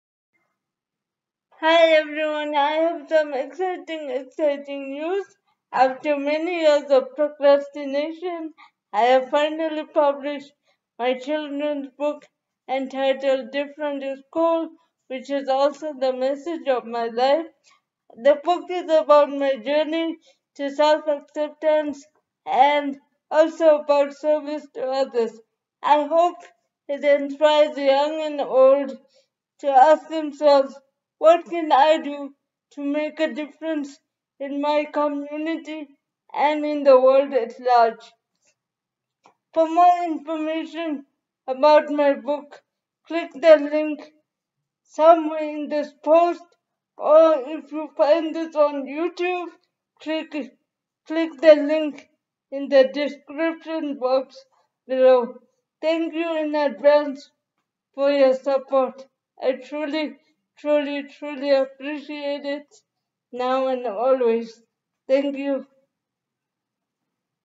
Different Is COOL out now on Amazon Kindle
On October 21, 2020 after many years of procrastination I finally published my children’s book entitled Different Is COOL which is about self-acceptance and service to others and I hope after reading it everybody will ask themselves what can I do to make a difference in my community and in the world at large.